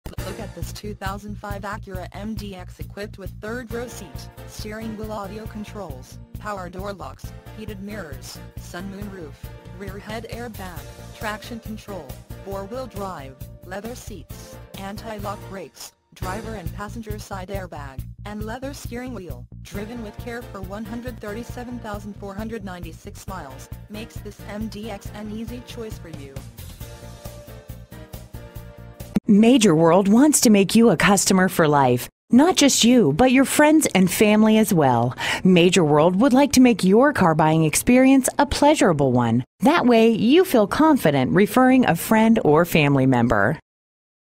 used Acura MDX NY New York 2005 located in Long Island City at Major World
This used Acura MDX NY New York 2005 is located in Long Island City and is offered by Major World and can be compared to any used Acura MDX :